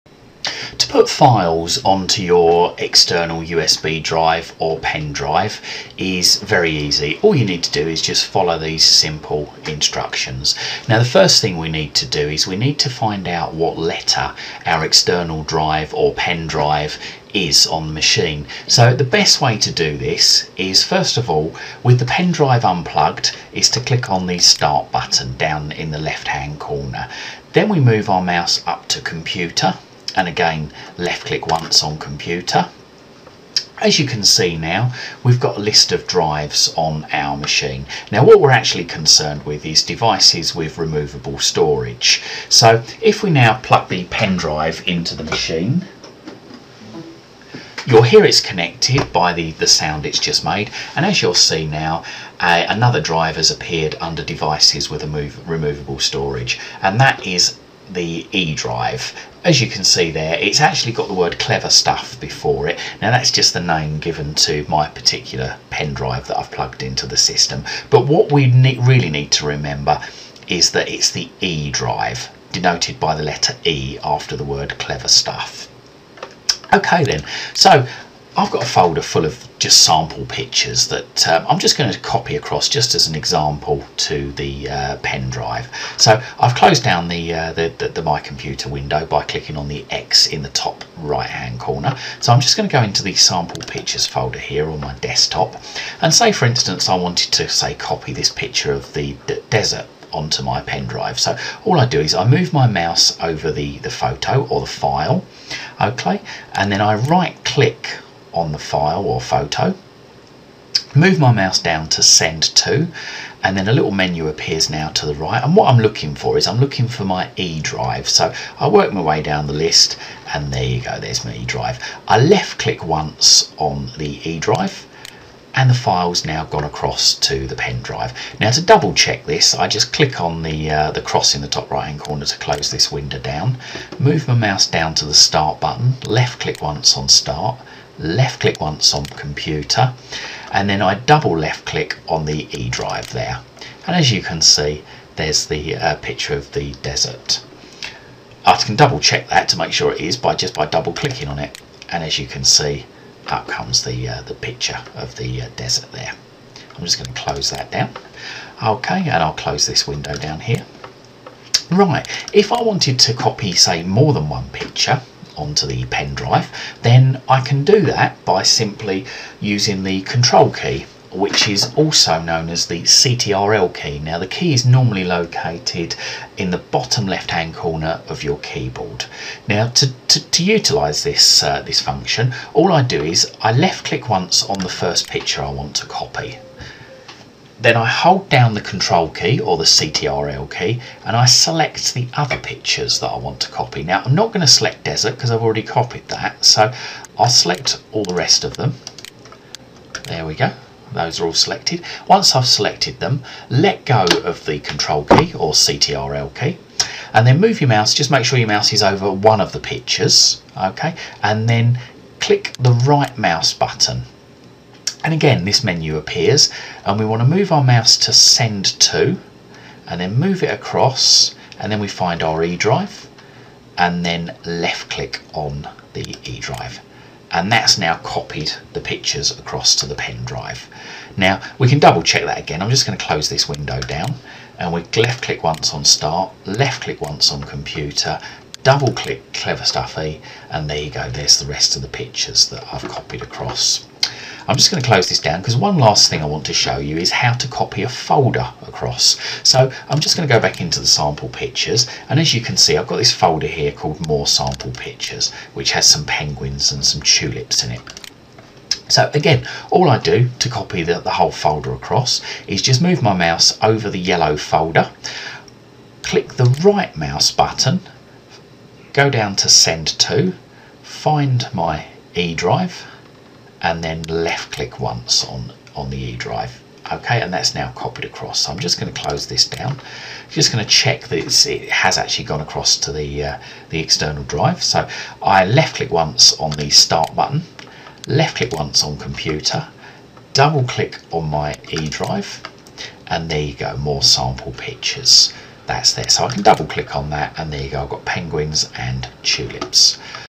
Copying files to an external drive
This video shows you how to copy files (ie Music, Pictures, Videos, Documents) to an external hard drive or usb pen drive in Windows, using the send to menu option.